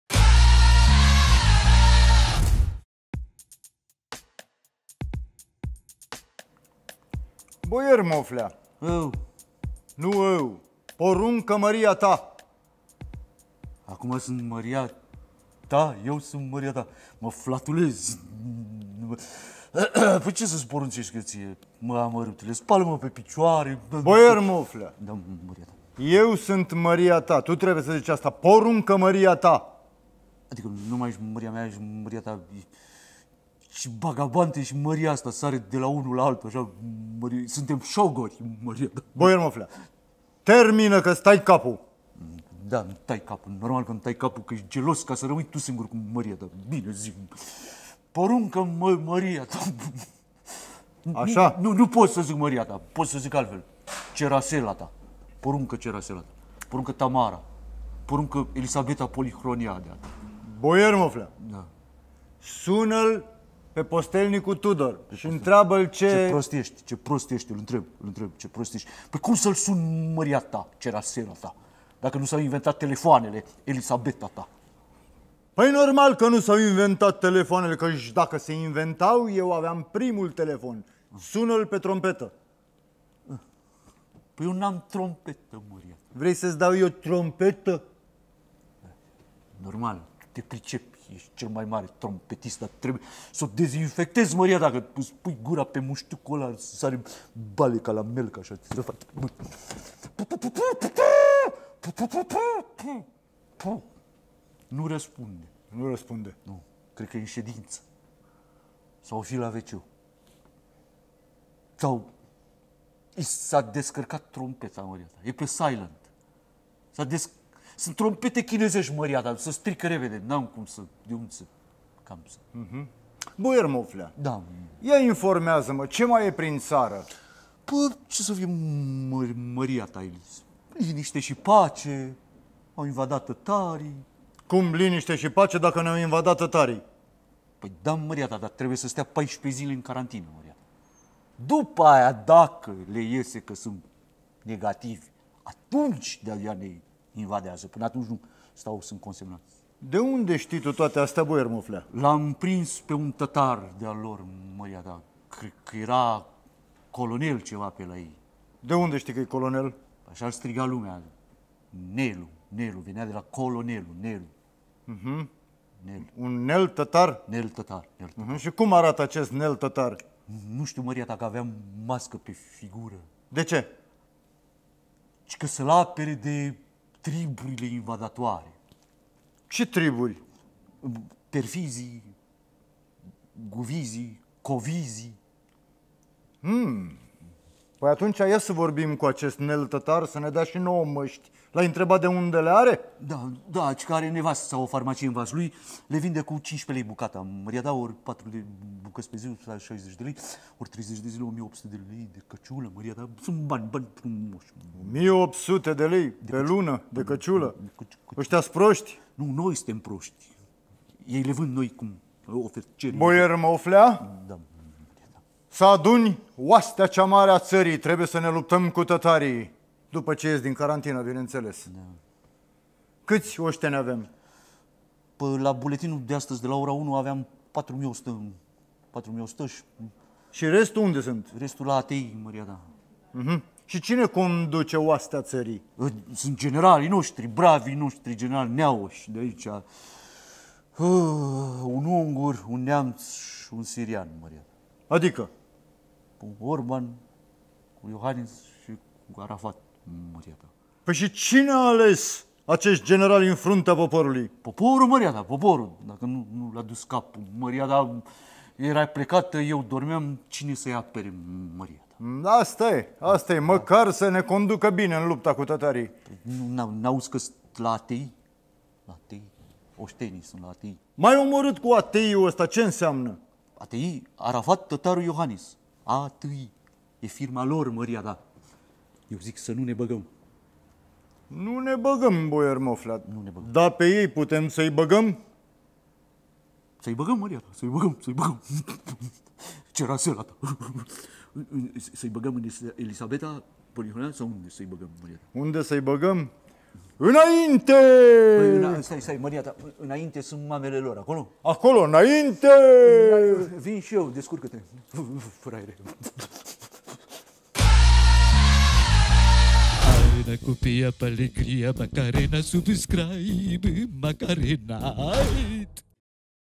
BOIER MOFLEA EP.1 - NELU TĂTARU
Dragi virtuali, vă propunem o miniserie cu super-eroul Boier Moflea. În episodul de astăzi el și Măria-Sa ( chiar, cum îl cheamă pe Măria -Sa???) trebuie să facă față amenințării tătarilor